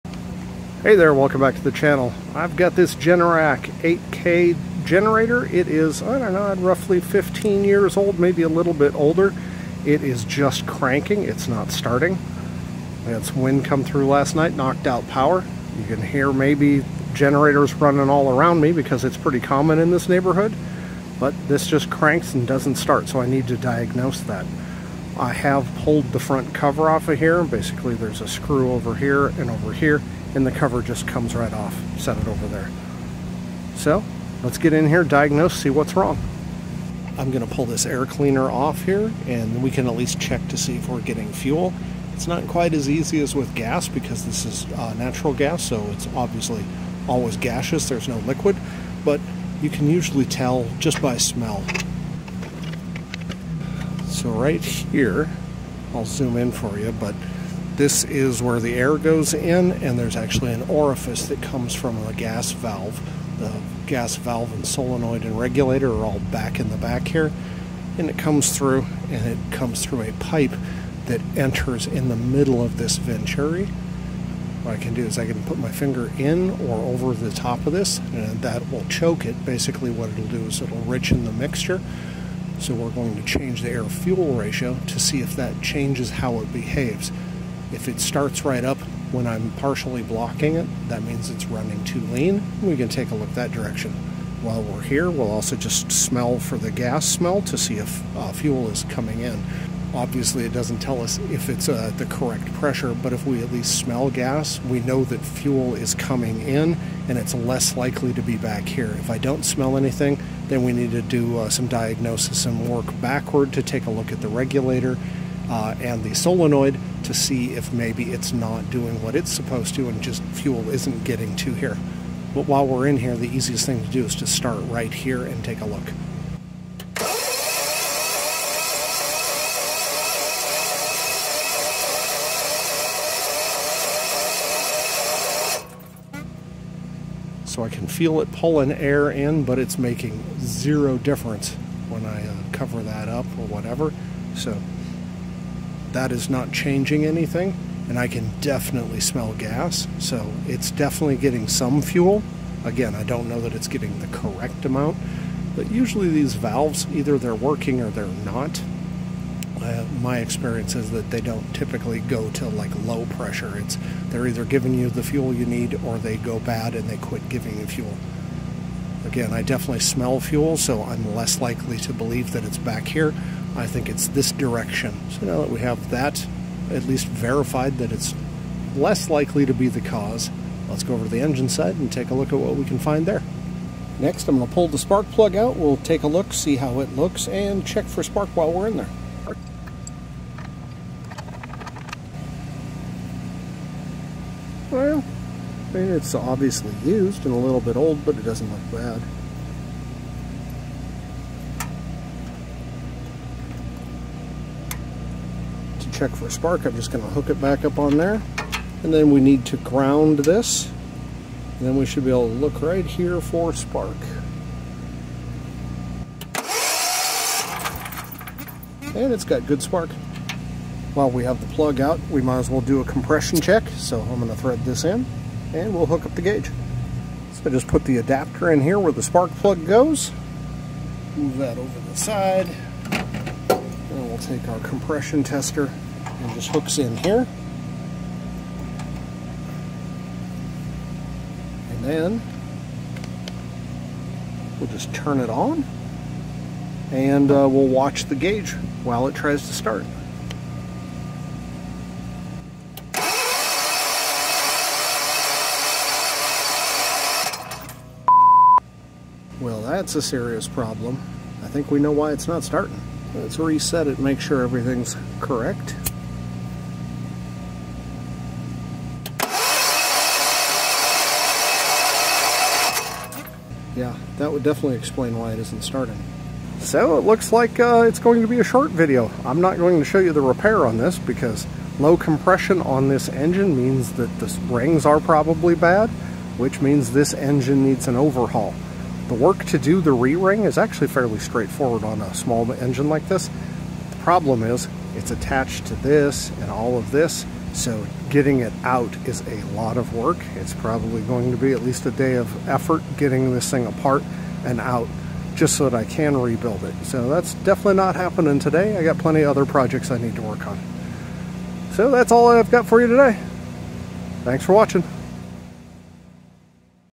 Generac Generator Cranks but Won't Start - A Quick Guide to Diagnosing the Problem
Generally, an engine needs fuel, air, and spark and it will run.  Today I'm doing a quick diagnosis on a Generac generator to see which it's missing and it turns out to be the 4th thing - compression.  Ugh.